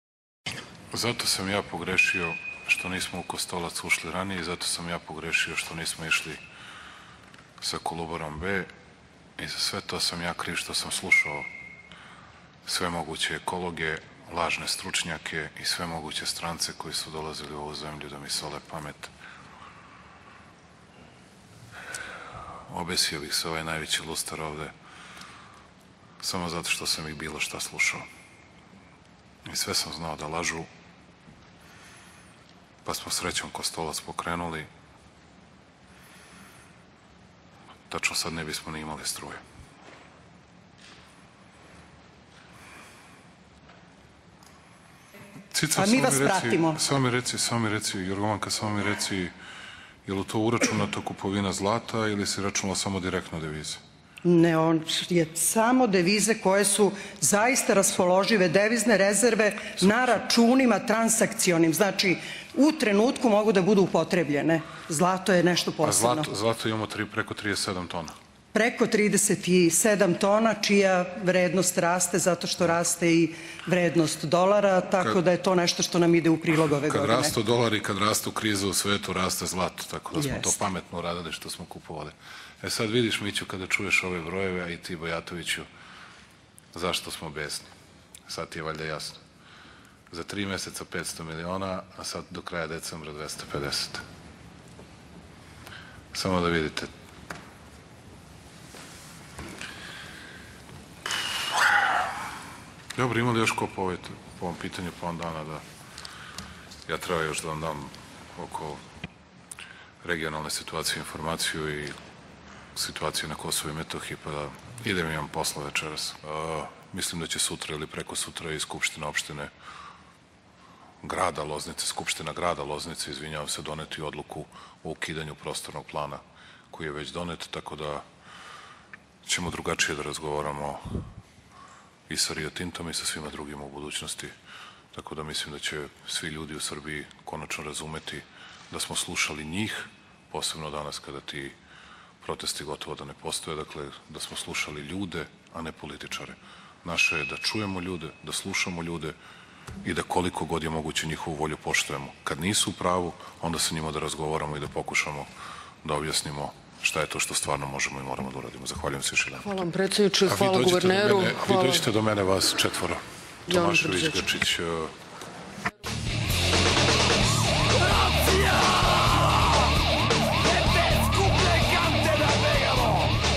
Kolaps energetskog sistema,Vučić hoće da se obesi na najviši luster,odgovornost Milorada Grčića
Greške EPS-a, Vlade i predsednika dovele do kolapsa energetskog sistema, poručeno na današnjoj javnoj sednici Vlade Srbije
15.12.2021